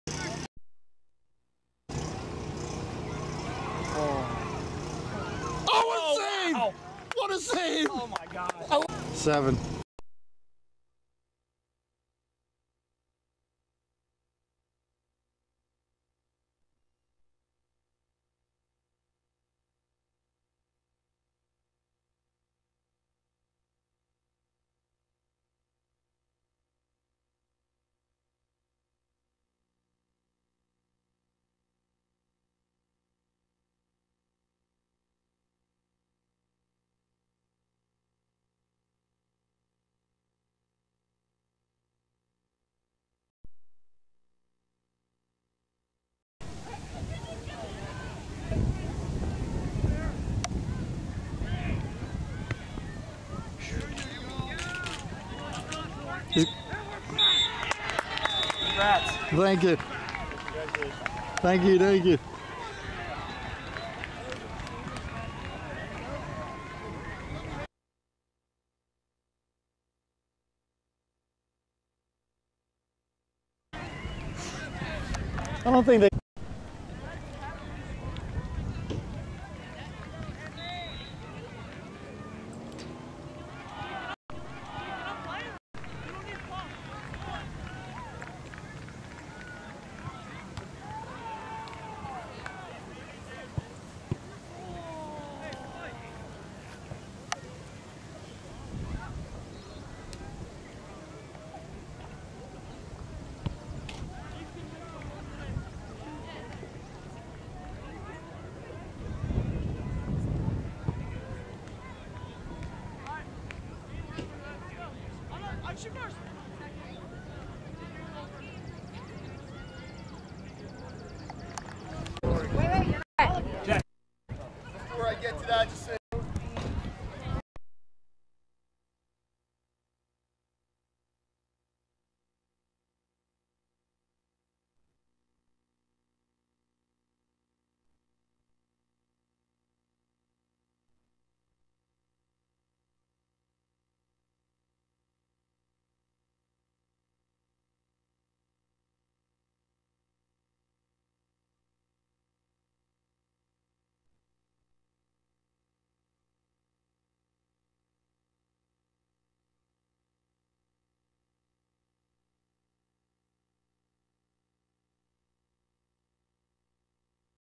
Patriot FC Brett huge save with less then 3 minutes left in State Cup win 1-0
Great Save